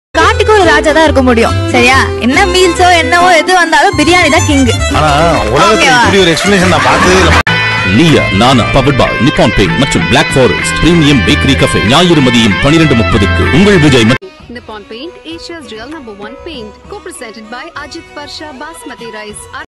Ajit Varsha Basmati Rice Chennai | Ajit Varsha Basmati Rice Ads | Ajit Varsha Vijay Tv Ads
Ajit Varsha Basmati Rice – It's love at first bite. The perfect rice nirvana. The extraordinary length of each grain is a natural example of food art. Enjoy the heightened sensory experience with the soft texture melting in your mouth. Known for its extraordinary length and slender shape, it has won favor in the homes and hearts of people across our land, as well as in international markets. It indeed is the extra special Basmati for extra special occasions. The grain is firm, fluffy in texture, and is matured for a period of 12 months; and these characteristics infuse it with some unique cooking properties.

 Name:  Senthil Kumar (Proprietor)
 Location:  # 1,2,3 Thandava Moorthy Street, Royapuram, (Near Harbour Gate No. 3)